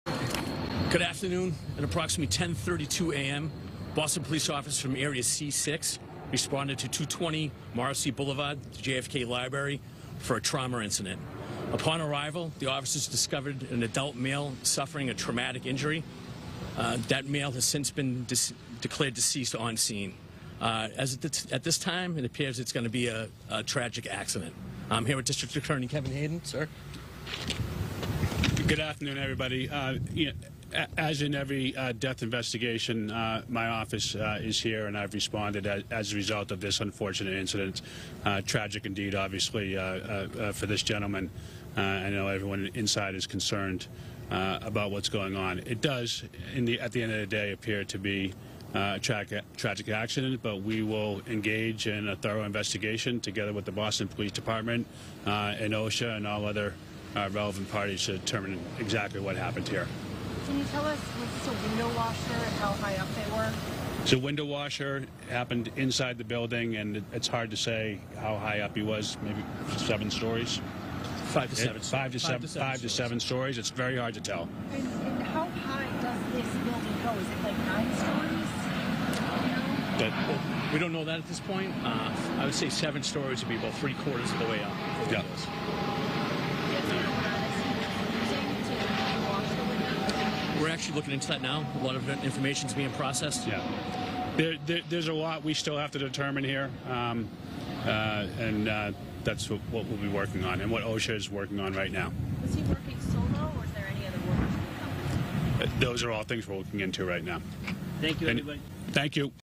Window washer dies in fall at JFK Library in Boston
A window washer died Wednesday morning when he fell at the John F. Kennedy Presidential Library and Museum in Boston.